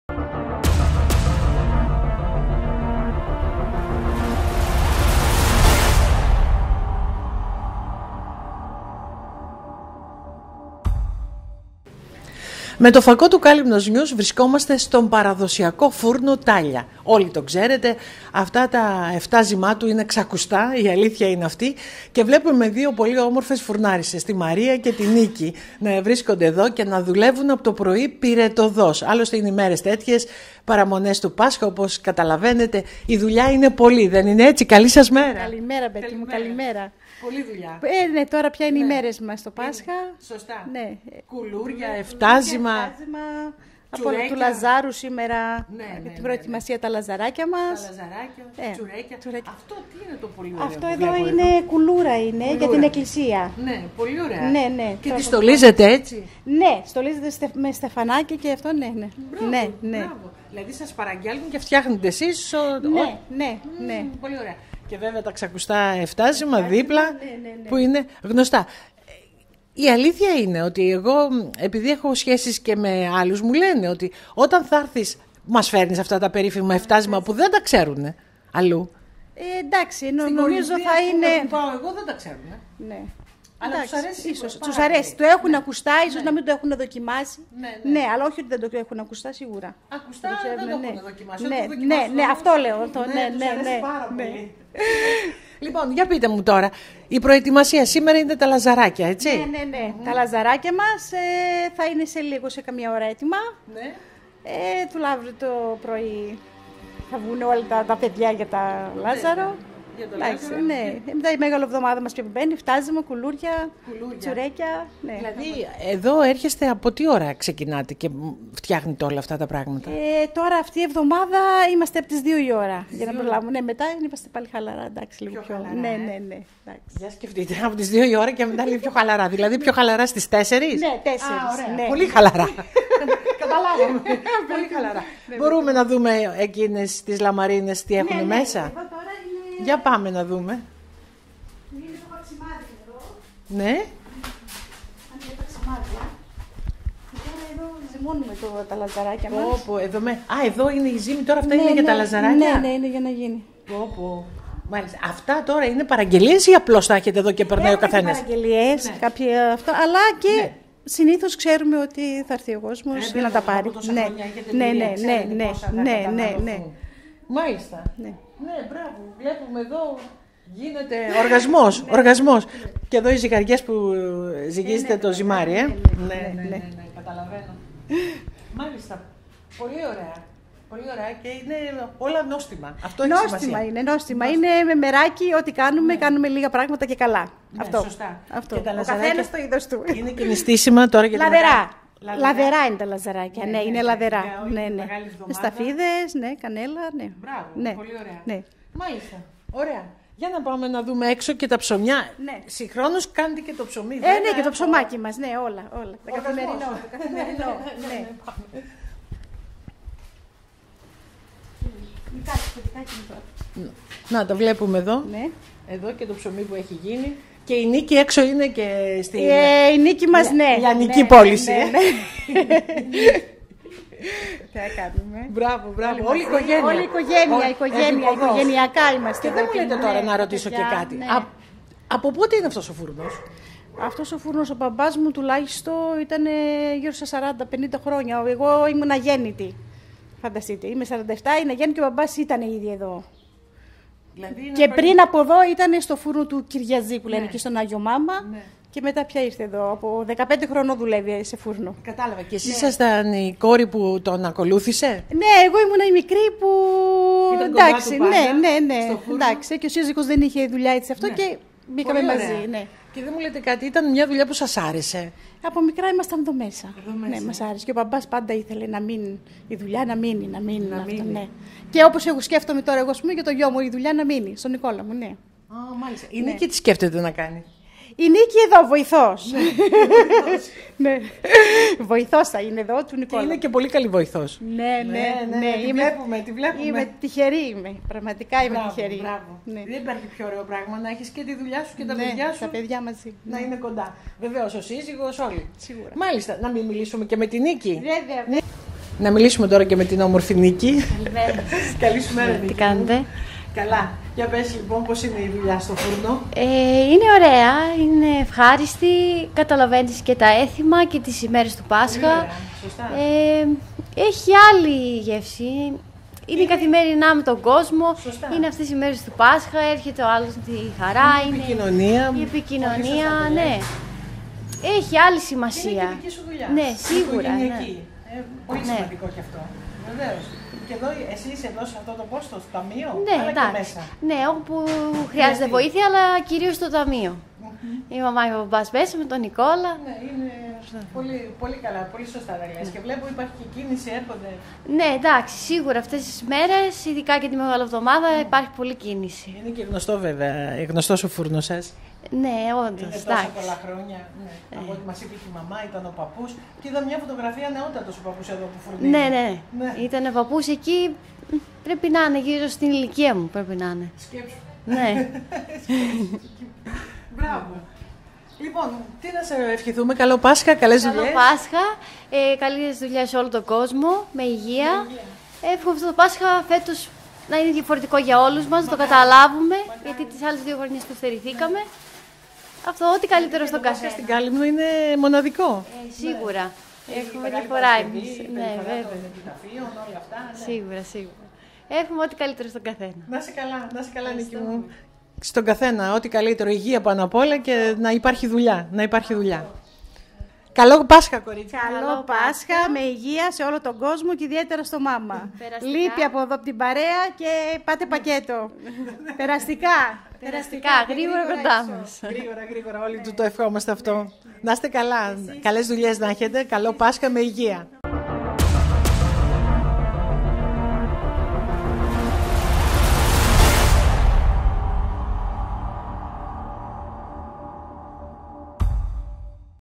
15-4-2022 Καλύμνικο «φτάζυμο» από τον παραδοσιακό φούρνο της οικογένειας Τάλια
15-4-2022   Καλύμνικο «φτάζυμο» από τον παραδοσιακό φούρνο της οικογένειας Τάλια